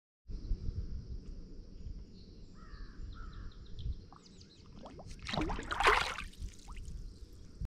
feedin catfish
hand feedin the catfish